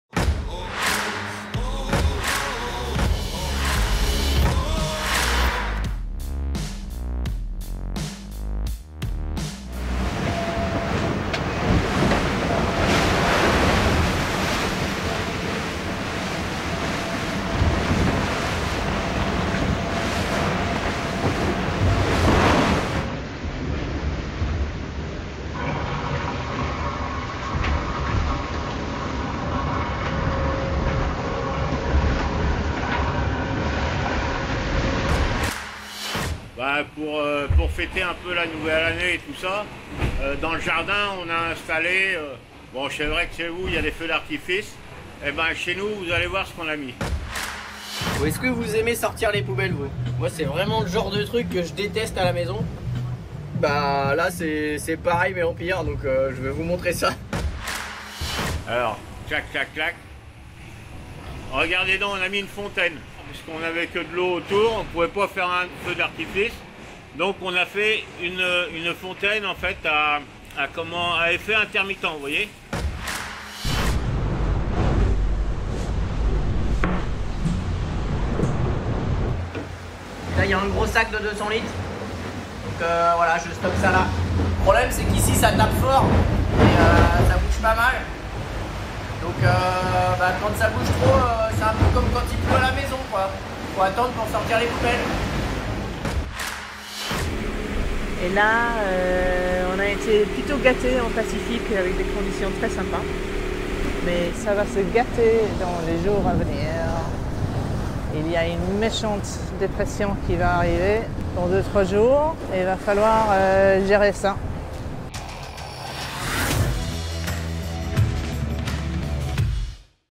#57 The race VG2020 - Today’s highlights
[Day 56 VG2020]
The rhythm of daily life is sometimes composed of small pleasures such as admiring a sunset, but sometimes less pleasant with the management of household chores: simple on land but complicated at sea.